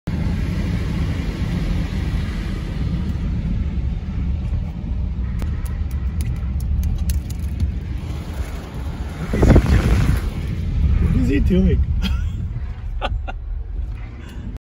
CRAZY BLACK BEAR VIDEO - running all over the road
Black bear running all over the road, kept crossing in front of me!